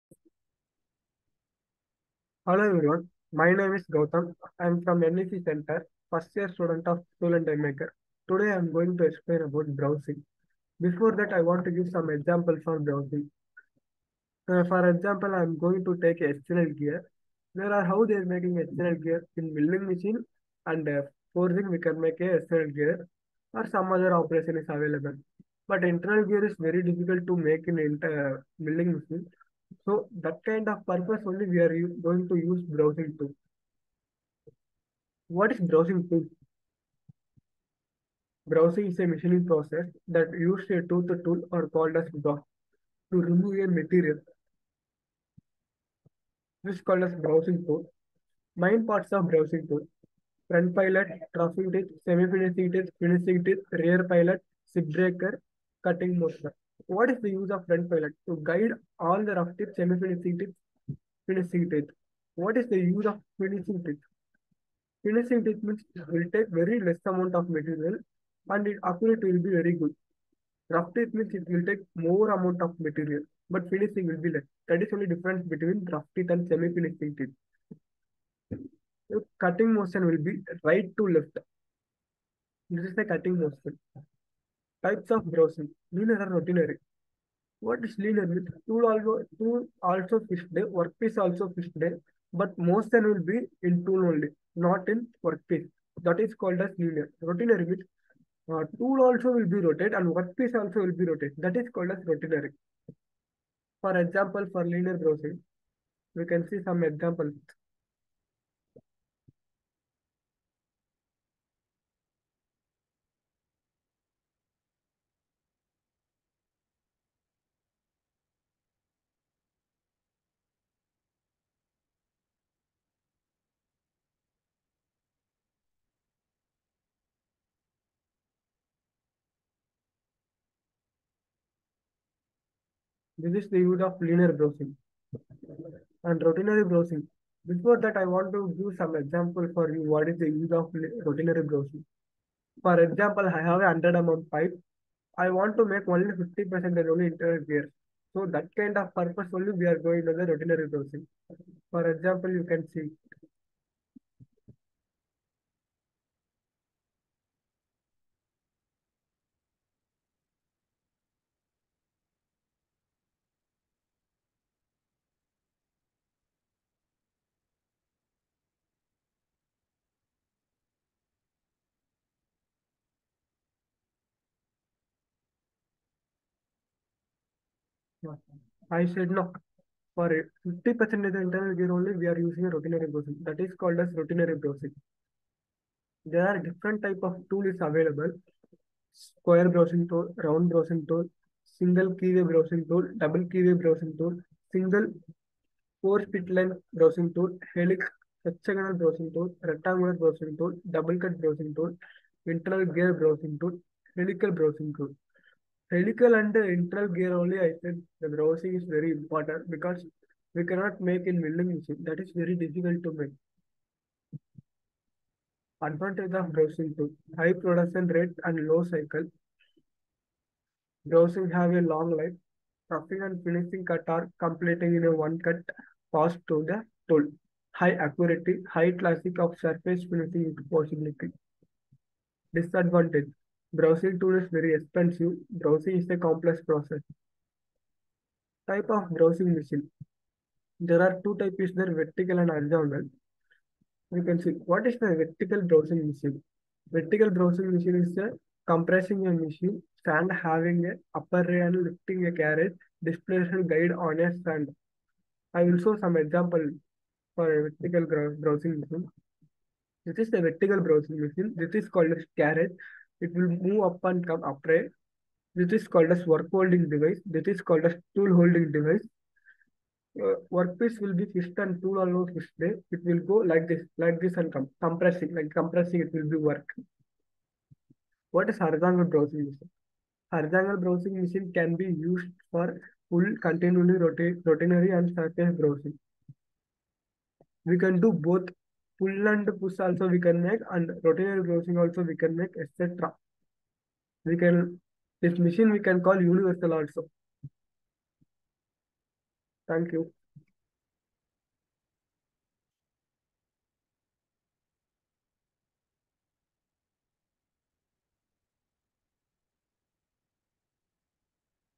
Broaching | Shop Talk | NTTF Electronic City (NEC) | May 2023
Gowtham presents an interesting shop talk on Broaching. He is currently pursuing his Diploma in Tool Engineering & Digital Manufacturing from NTTF Electronic City, Bangalore.